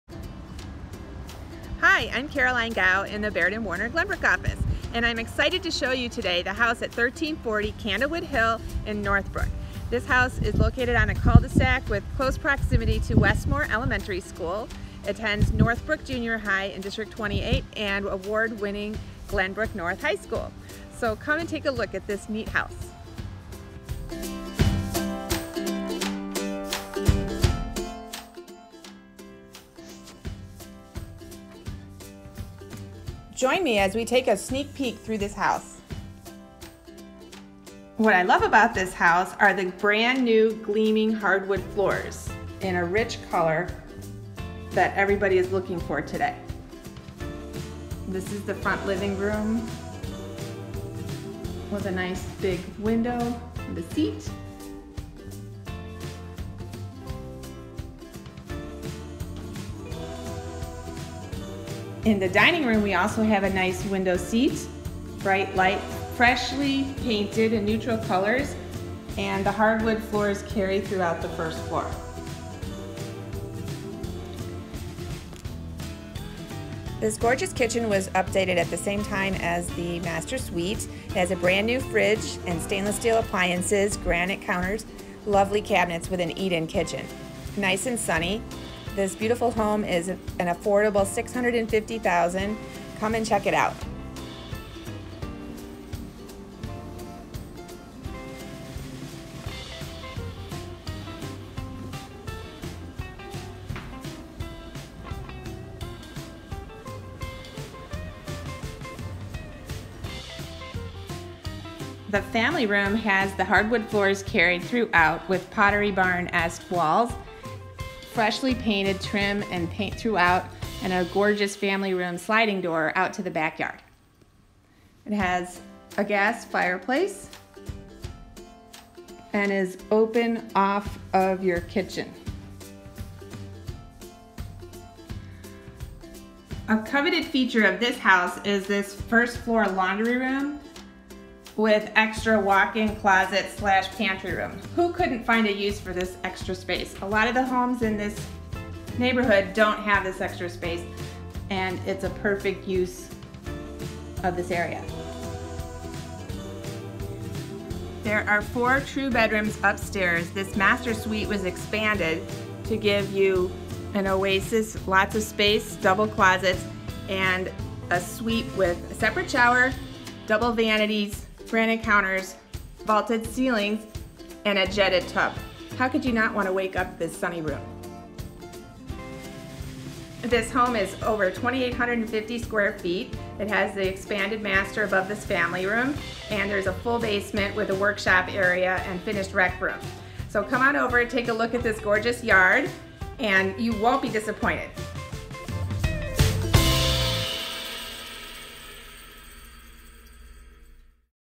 Turnkey Ready Property Video Northbrook Dist 28 Caroline Gau, Baird & Warner
Brand new hardwood floors, freshly painted in neutral colors this 4 Bedroom, 2.5 bath home with expanded master suite has had thousands invested in updates. You spoke, we listened. Be impressed with this turnkey beauty in Westmoor Elementary School in Dist. 28.
Conveniently located on cul du sac near K-5 school and in Northbrook Jr High and award-winning Glenbrook North High School. Check out this sneak peek at this beautiful home.